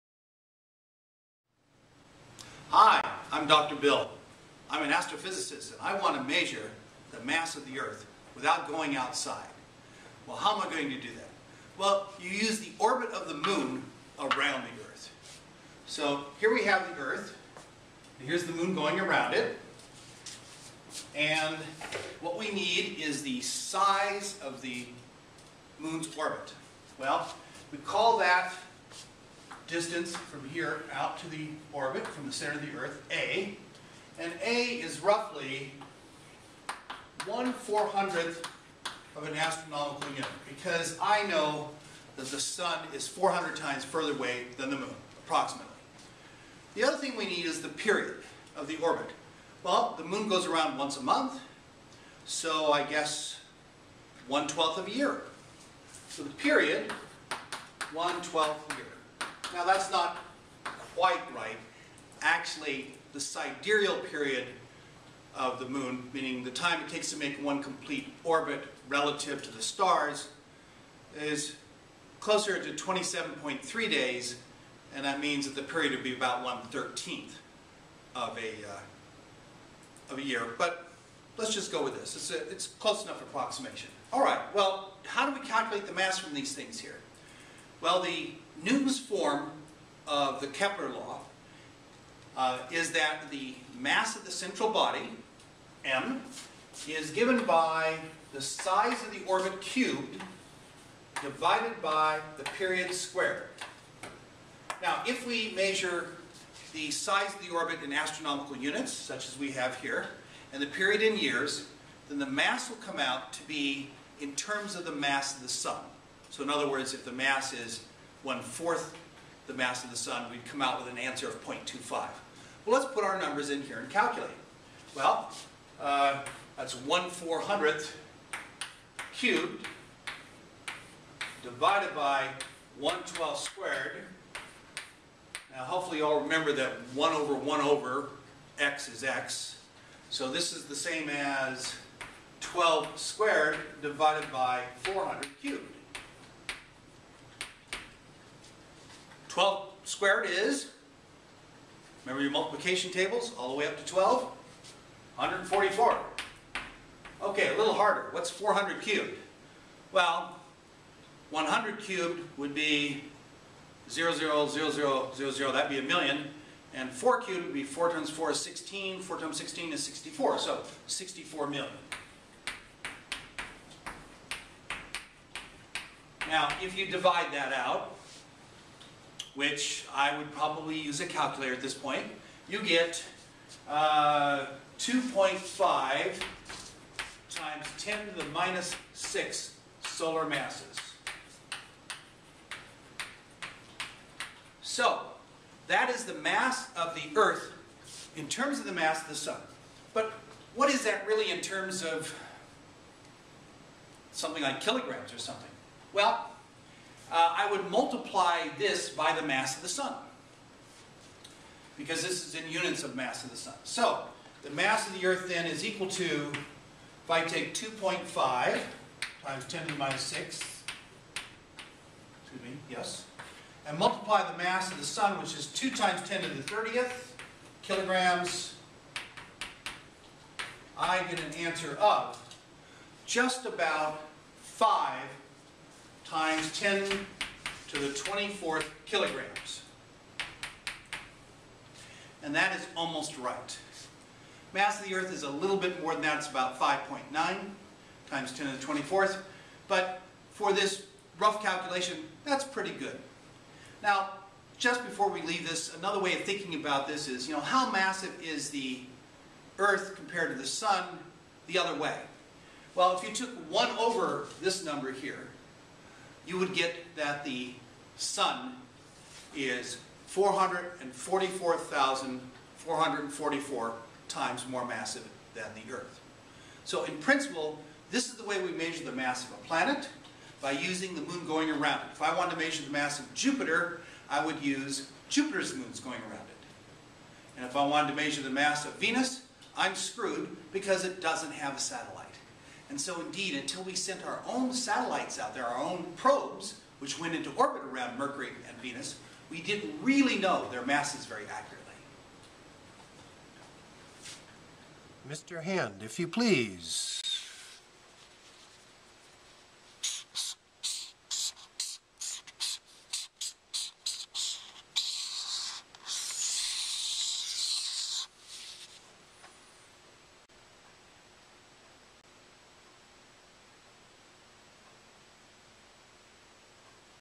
Measuring the Mass of the Earth from the Orbit of the Moon
Application of Newton's form of Kepler's 3rd Law.  The mass of a central body can be calculated from the orbital period and orbital radius of its moon.